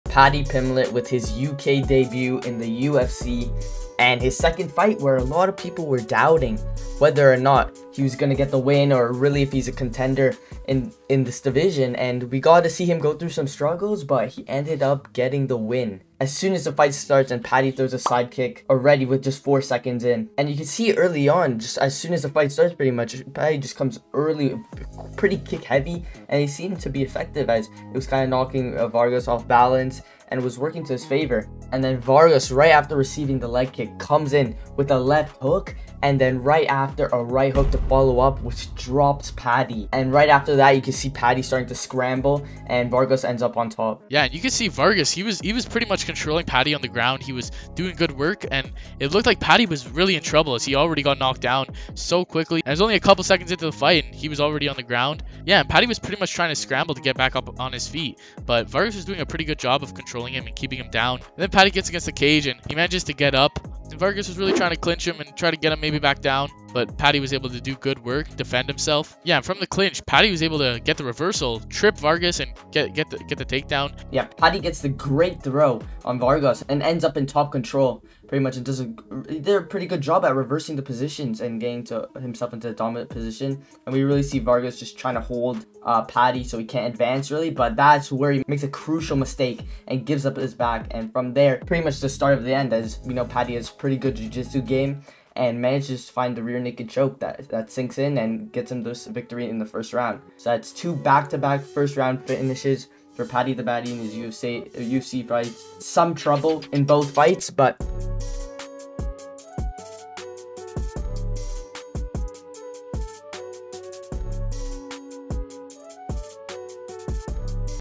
Paddy Pimblett vs Rodrigo Vargas FULL FIGHT RECAP - What REALLY Happened on UFC London
In this video we breakdown and analysis up and coming star Paddy Pimblett's fight against Rodrigo Vargas!